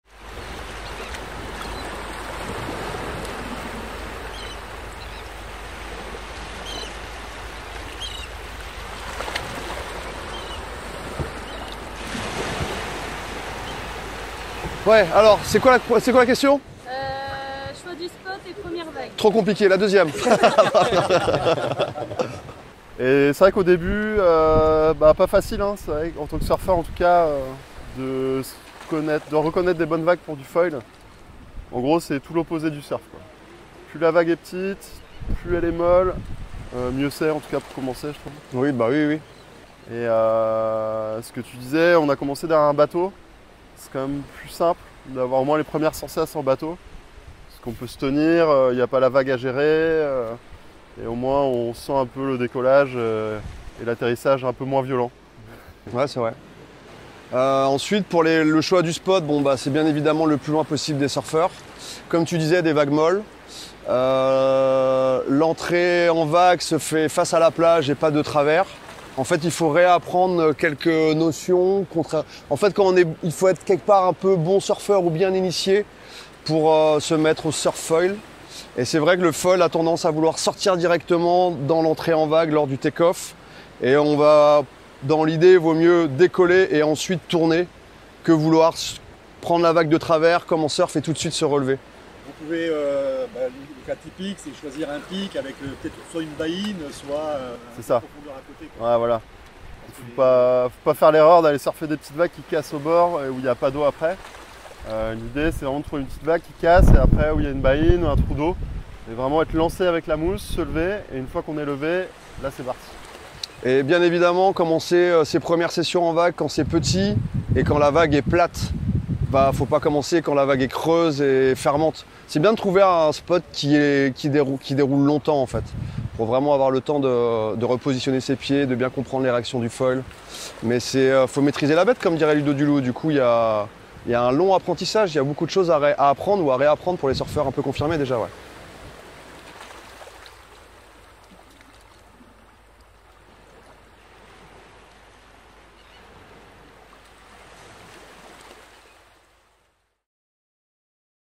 [TUTOS SURF FOIL] CHOIX DU SPOT ET PREMIERES VAGUES
Les @Lostintheswell0 vous donne des conseils pour choisir votre spots et prendre vos premières vagues en surf foil !⬇⬇⬇